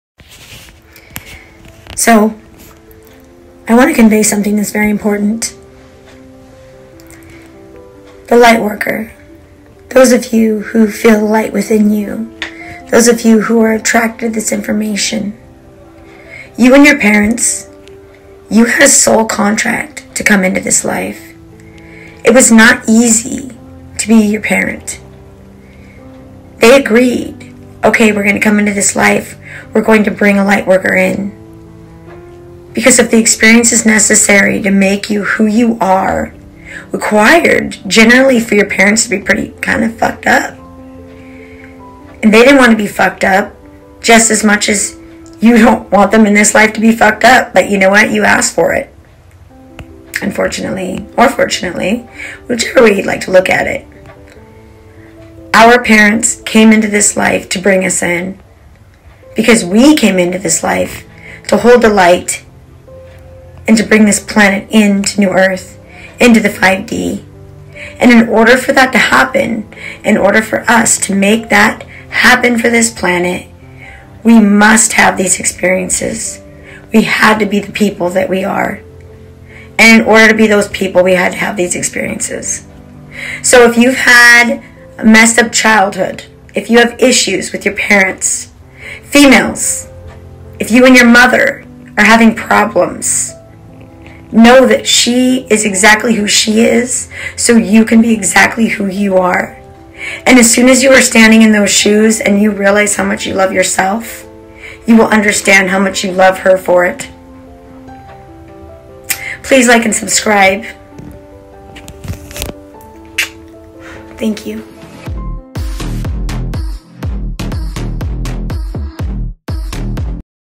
The Most Powerful Message for Light Workers in 2024!
As light workers, we have had a soul contract with our parents with those souls who are playing the role of our parents in this life and that contract involved, making sure that we had the experiences necessary for us to be the people that we are for us to be able to hold the light for us to experience the contrast and polarity necessary to be the people that stand in the shoes who are capable of transitioning into the 5D this wasn't an easy job and that's why it required a soul contract for it to be completed so before you become angry or resentful with your parents, understand that they are exactly who they are so you can be exactly who you are.￼